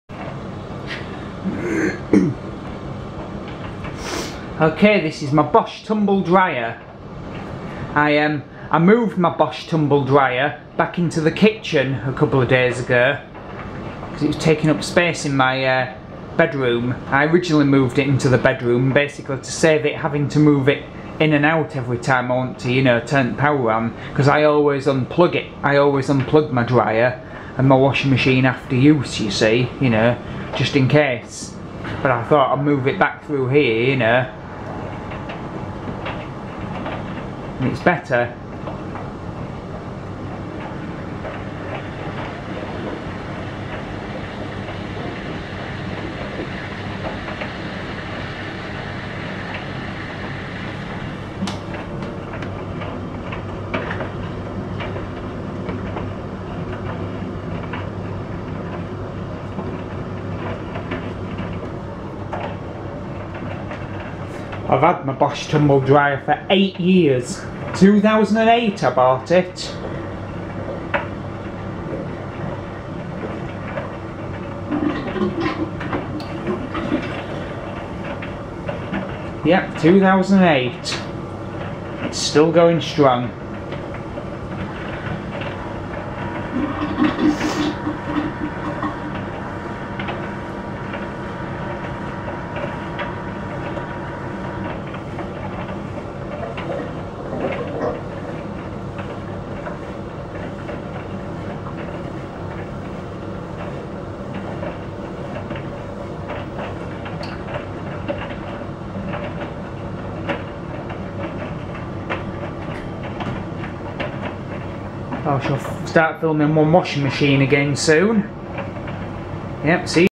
My bosch Tumble dryer drying some towels
This is my bosch tumble dryer which i have owned for the past 8 years. The handle has come off as you can see but it still works perfectly.